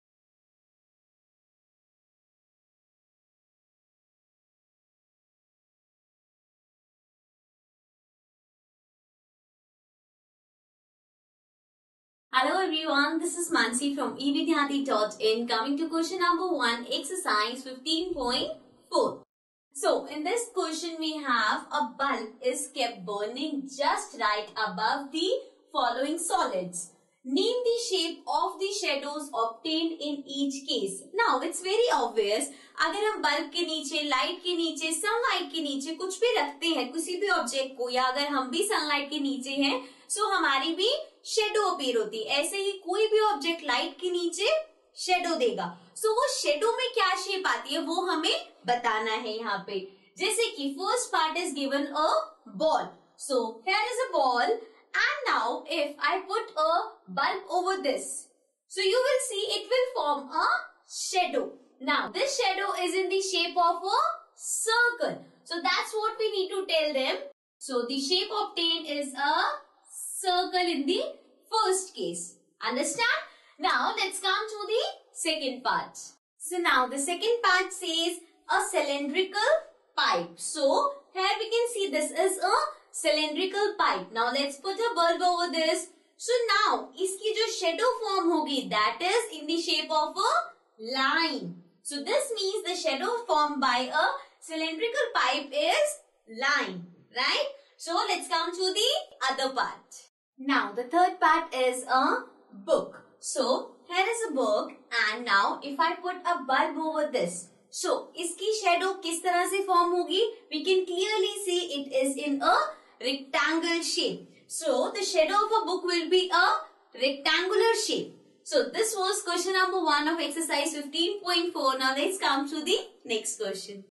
Q 1, Ex 13.4 - Visualising Solid Shapes, Chapter 13, Maths Class 7th - NCERT, New Syllabus 2023 CBSE
Visualising Solid Shapes - Solution for Class 7th mathematics, NCERT  solutions for Class 7th Maths. Get Textbook solutions for maths from evidyarthi.in